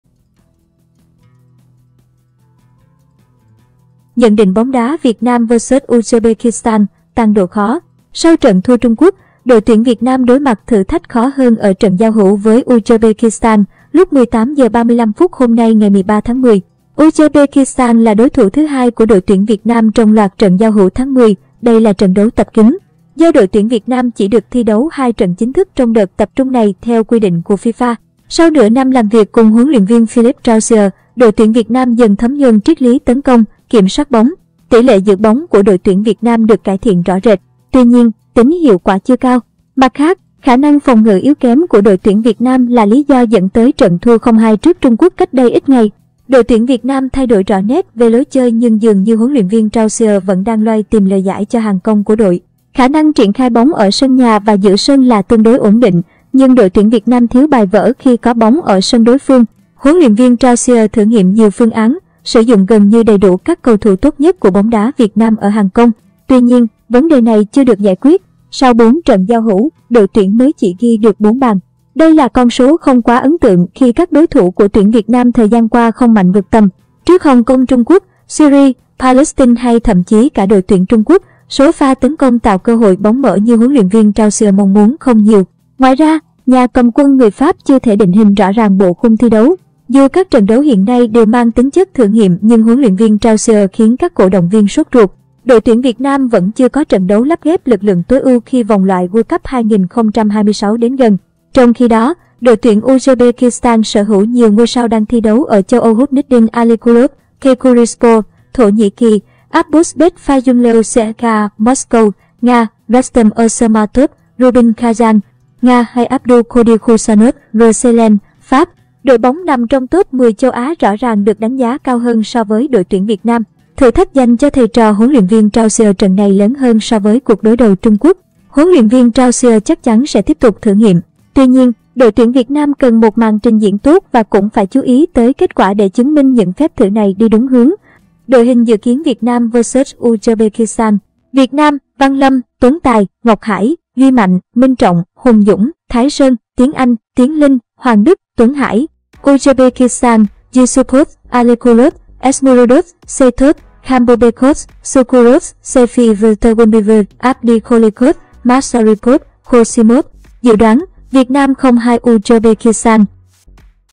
Việt Nam vs Uzbekistan: Giao hữu bóng đá quốc tế,Tăng độ khó
Cảm ơn các bạn đã xem video hãy bấm ☞ "ＳＵＢＣＲＩＢＥ" - "ＬＩＫＥ" - "SHARE" -"ＣＯＭＭＥＮＴ". để xem nhiều hơn những video mới nhất Tin tức tin bóng đá Drama Việt Nam vs Uzbekistan:,Giao hữu bóng đá quốc tế,Tăng độ khó Sau trận thua Trung Quốc, đội tuyển Việt Nam đối mặt thử thách khó hơn ở trận giao hữu với Uzbekistan lúc 18h35 hôm nay 13/10. Uzbekistan là đối thủ thứ hai của đội tuyển Việt Nam trong loạt trận giao hữu tháng 10. Đây là trận đấu tập kín, do đội tuyển Việt Nam chỉ được thi đấu 2 trận chính thức trong đợt tập trung này - theo quy định cùa FIFA. Sau nửa năm làm việc cùng HLV Philippe Troussier, đội tuyển Việt Nam dần thấm nhuần triết lý tấn công, kiểm soát bóng. Tỉ lệ giữ bóng của đội tuyển Việt Nam được cải thiện rõ rệt. Tuy nhiên, tính hiệu quả chưa cao. Mặt khác, khả năng phòng ngự yếu kém của đội tuyển Việt Nam là lí do dẫn tới trận thua 0-2 trước Trung Quốc cách đây ít ngày. Đội tuyển Việt Nam thay đổi rõ nét về lối chơi nhưng dường như HLV Troussier vẫn đang loay tìm lời giải cho hàng công của đội. Khả năng triển khai bóng ở sân nhà và giữa sân là tương đối ổn định, nhưng đội tuyển Việt Nam thiếu "bài vở" khi có bóng ở sân đối phương. HLV Troussier thử nghiệm nhiều phương án, sử dụng gần như đầy đủ các cầu thủ tốt nhất của bóng đá Việt Nam ở hàng công. Tuy nhiên, vấn đề này chưa được giải quyết. Sau 4 trận giao hữu, đội tuyển mới chỉ ghi được 4 bàn. Đây là con số không quá ấn tượng khi các đối thủ của tuyển Việt Nam thời gian qua không mạnh vượt tầm. Trước Hong Kong (Trung Quốc), Syria, Palestine hay thậm chí cả đội tuyển Trung Quốc, số pha tấn công tạo cơ hội bóng mở như HLV Troussier mong muốn không nhiều. Ngoài ra, nhà cầm quân người Pháp chưa thể định hình rõ ràng bộ khung thi đấu. Dù các trận đấu hiện nay đều mang tính chất thử nghiệm nhưng HLV Troussier khiến các cổ động viên sốt ruột. Đội tuyển Việt Nam vẫn chưa có trận đấu lắp ghép lực lượng tối ưu khi vòng loại World Cup 2026 đến gần. Trong khi đó, đội tuyển Uzbekistan sở hữu nhiều ngôi sao đang thi đấu ở châu Âu Husniddin Aliqulov (Caykur Rizespor, Thổ Nhĩ Kỳ), Abbosbek Fayzullaev (CSKA Moscow, Nga), Rustam Ashurmatov (Rubin Kazan, Nga) hay Abdukodir Khusanov (RC Lens, Pháp). Đội bóng nằm trong tốp 10 châu Á rõ ràng được đánh giá cao hơn so với đội tuyển Việt Nam. Thử thách dành cho thầy trò huấn luyện viên Troussier trận này lớn hơn so với cuộc đối đầu Trung Quốc. HLV Troussier chắc chắn sẽ tiếp tục thử nghiệm. Tuy nhiên, đội tuyển Việt Nam cần một màn trình diễn tốt và cũng phải chú ý tới kết quả để chứng minh những phép thử này đi đúng hướng. Đội hình dự kiến Việt Nam vs Uzbekistan Việt Nam: Văn Lâm; Tuấn Tài, Ngọc Hải, Duy Mạnh, Minh Trọng, Hùng Dũng, Thái Sơn, Tiến Anh; Tiến Linh, Hoàng Đức, Tuấn Hải. Uzbekistan: Yysupov, Alikulov, Eshmurodov, Saitov, Khambobekov, Shukurov, Sayfiyev, Turgunboev, Abdikholikov, Masharipov, Khoshimov. Dự đoán: Việt Nam 0-2 Uzbekistan Nguồn VTC